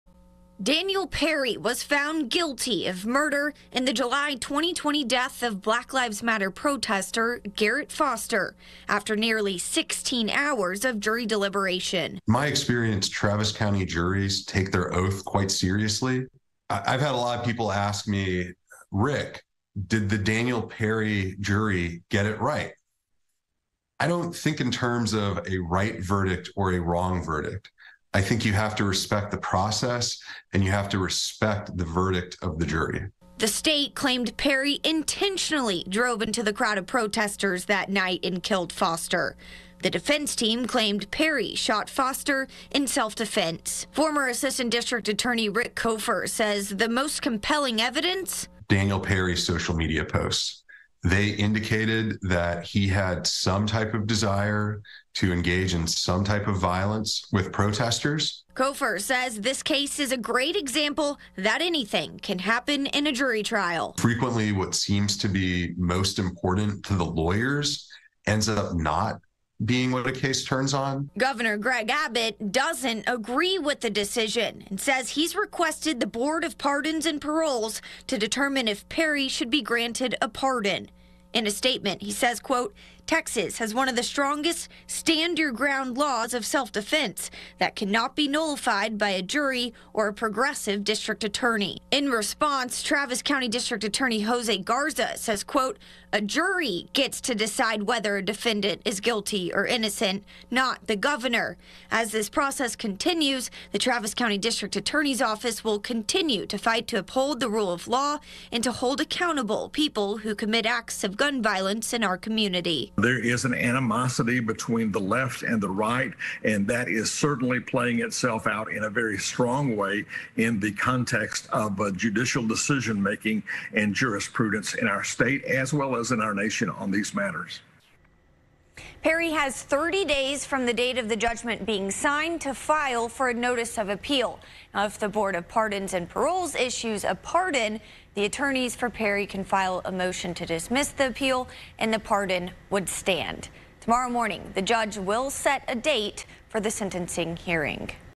Travis County DA reacts to Abbott’s call for pardon of Daniel Perry | FOX 7 Austin
On Saturday, Gov. Abbott requested the Board of Pardons and Paroles to look into pardoning Sgt. Daniel Perry. Perry was convicted of murder in the 2020 shooting death of Black Lives Matter protester Garrett Foster.